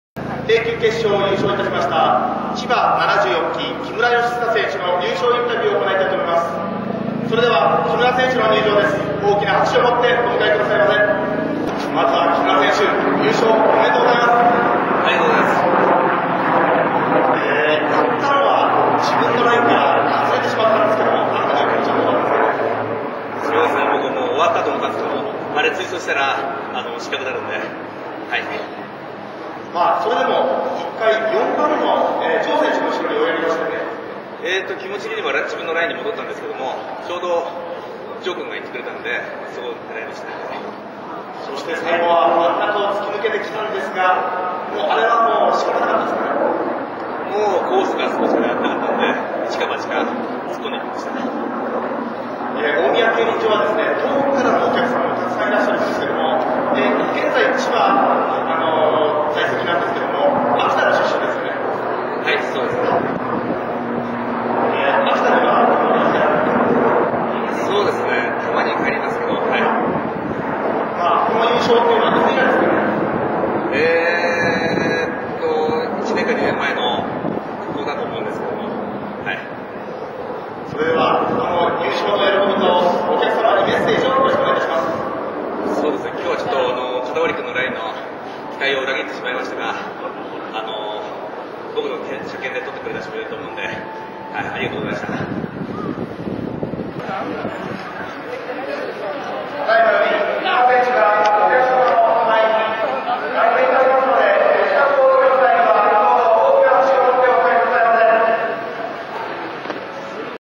2011.02.17 大宮競輪 A級決勝10R 優勝者インタビュー
第40回 東京中日スポーツ杯争奪戦 後節 F1
大宮競輪 A級決勝10R
優勝者　木村　悦久選手(千葉 74期 A1)